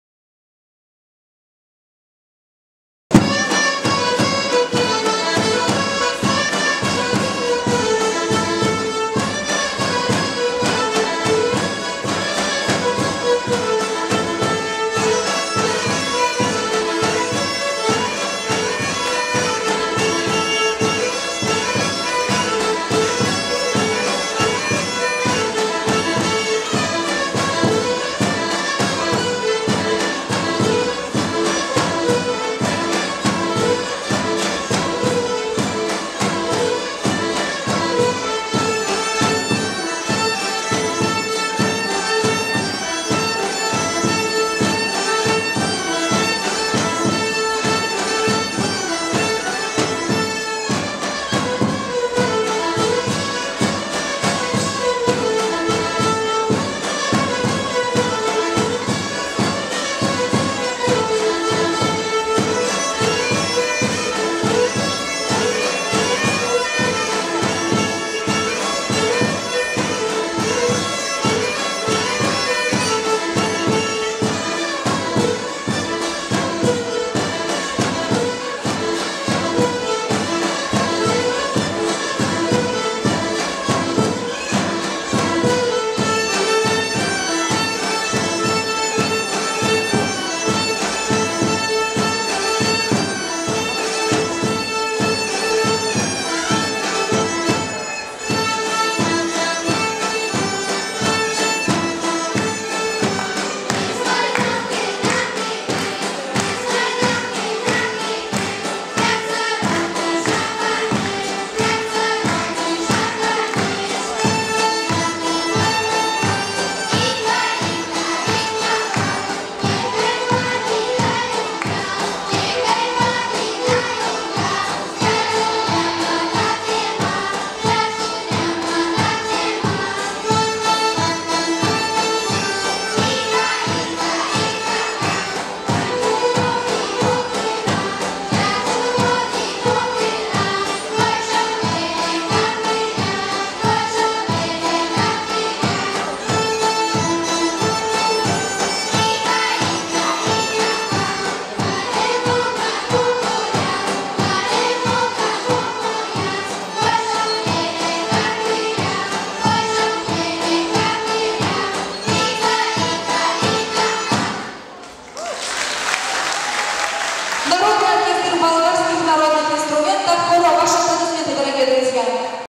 Народный оркестр болгарской музыки «Хоро» - с. Заря
Участники отборочного конкурса "Феерия талантов Бессарабии" Народный оркестр болгарской музыки «Хоро»."Попури на тему болгарских народных песен. (с.Заря, Саратский район).